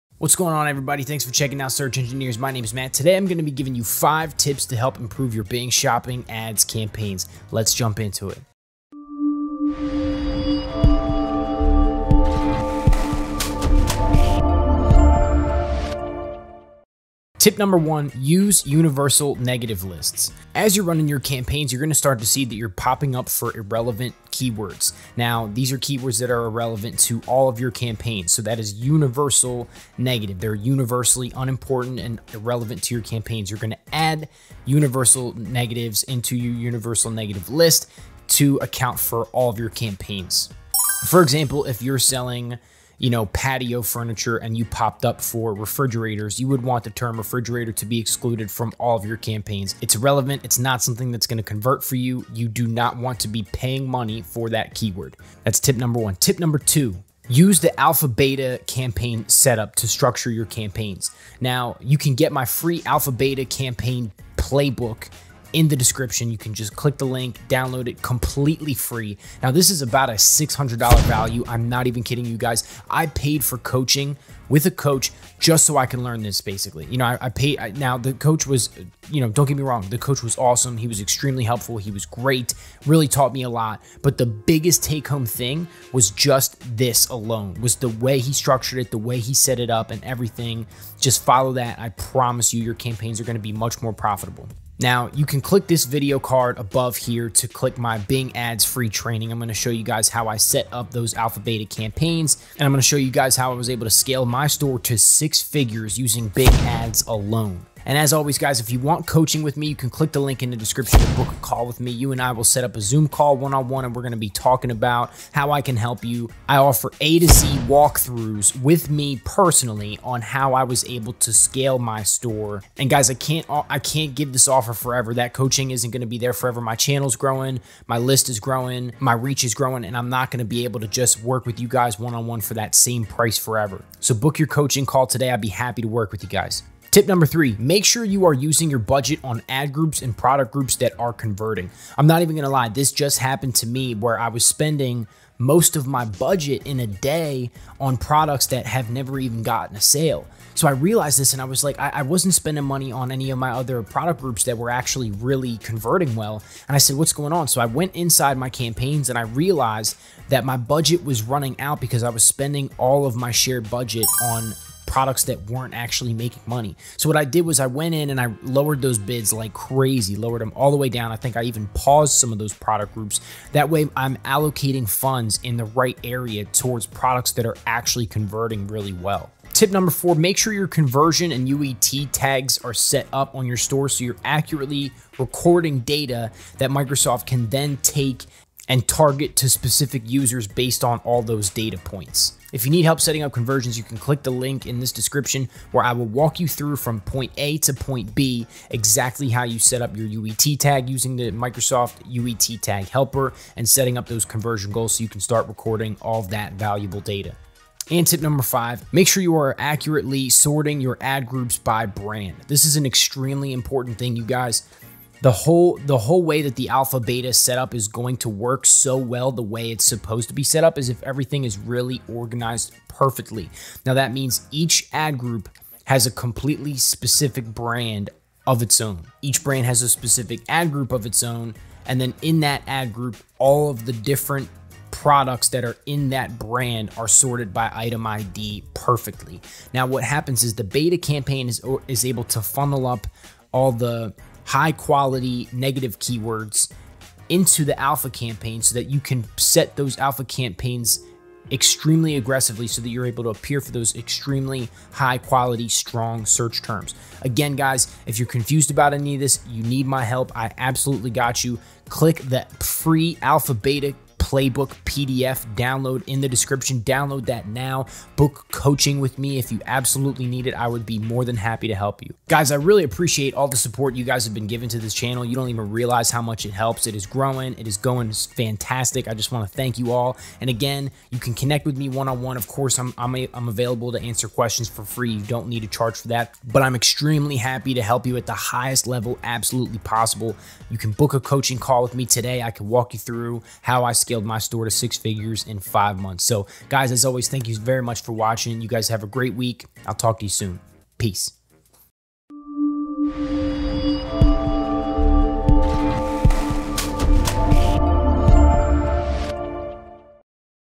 Bing Shopping Ads: 5 Tips to BOOST Conversions!
In this video, I show you 5 MAJOR tips to help boost your sales in Bing Shopping Ads.

Lots of people overlook these 5 things, don't be one of those people!

Microsoft Advertising is always making updates to their platform, but these things have been consistent for as long as I've been advertising on their platform.

Bing ads dropshipping is a fantastic way to generate tons of revenue. I walk you through step by step to set up 6 figure campaigns FAST in my coaching program.